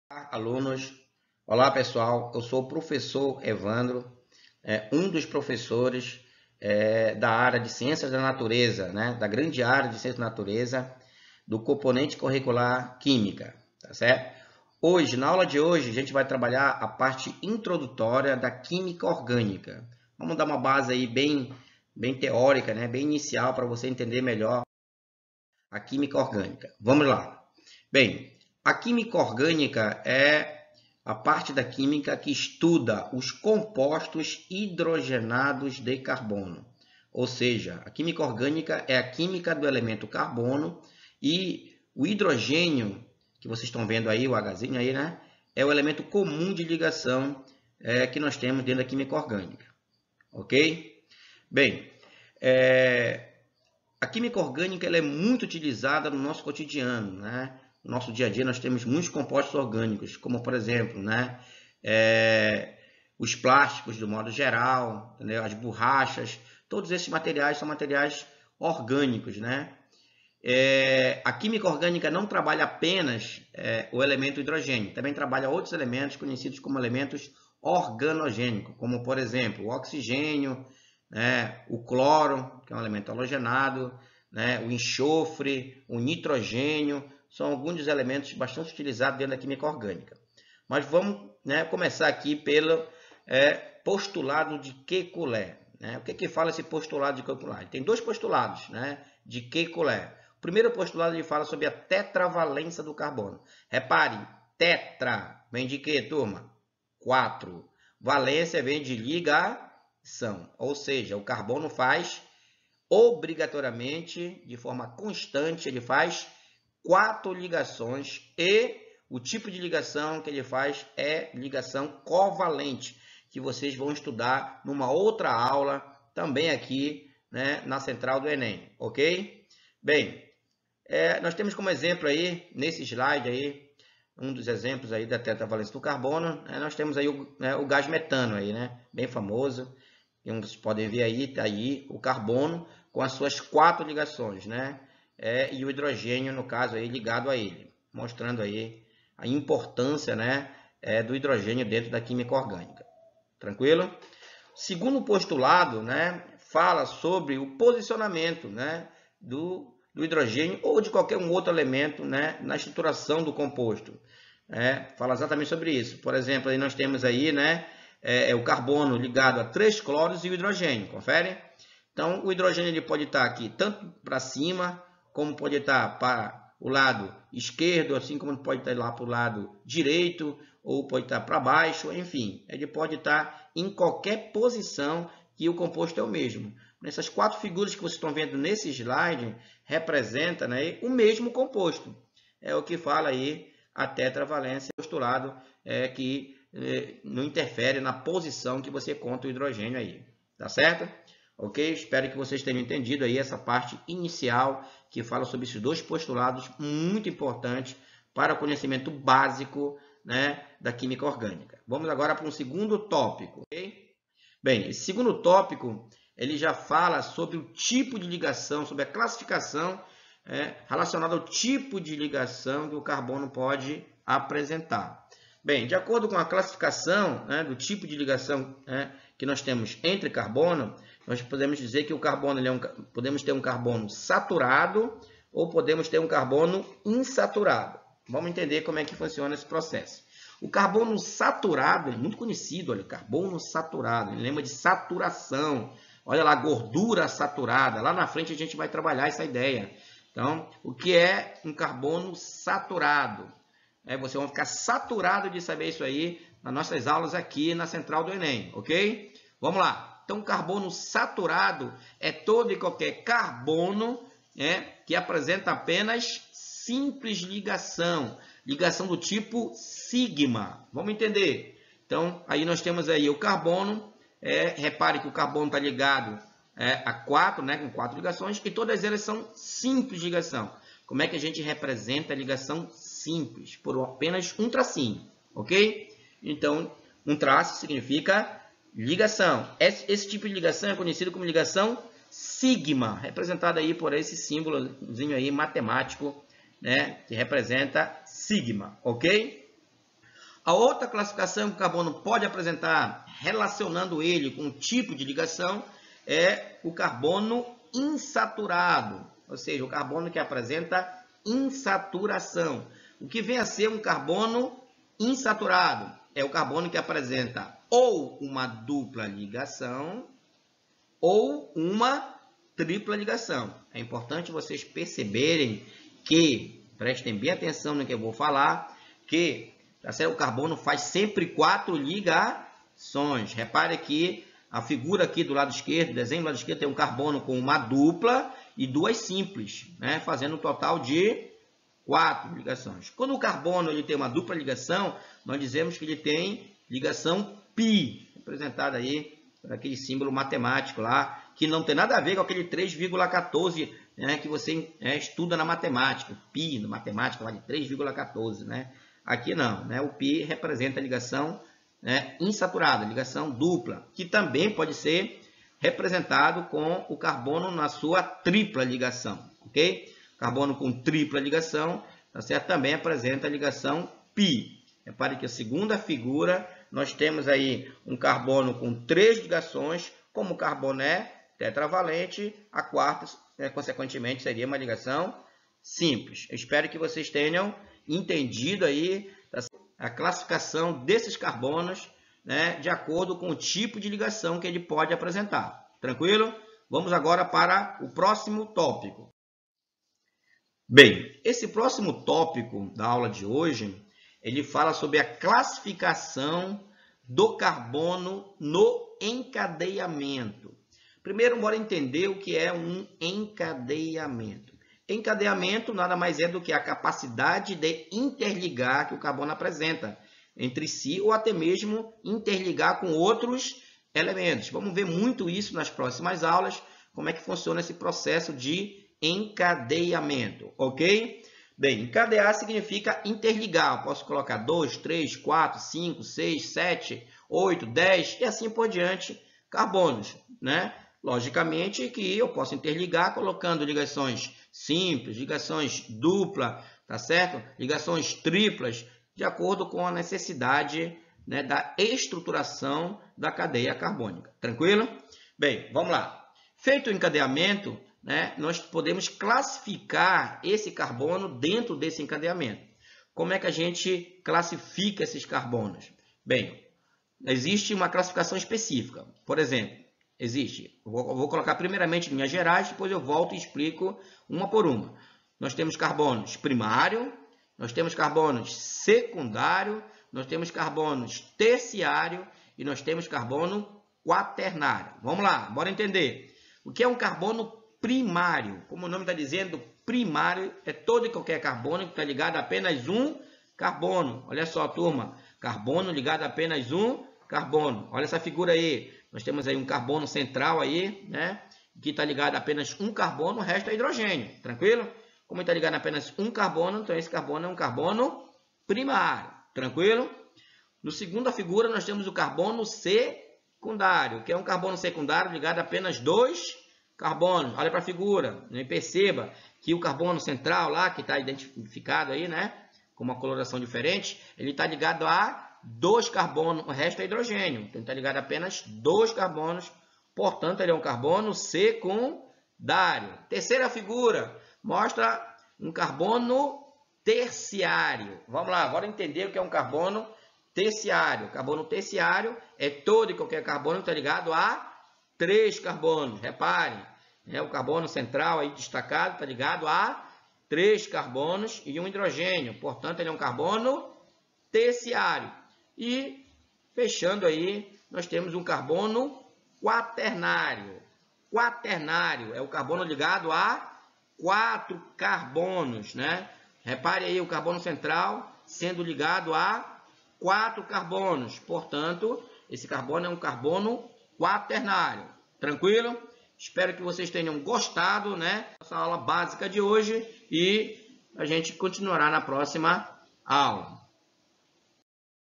Química Orgânica Química com professor Evandro
Ciências da Natureza e suas Tecnologias. Revisão de Química Orgânica.
Aula do dia 2 de maio de 2020.
Siga nosso insta @centraldoenem_ap ;)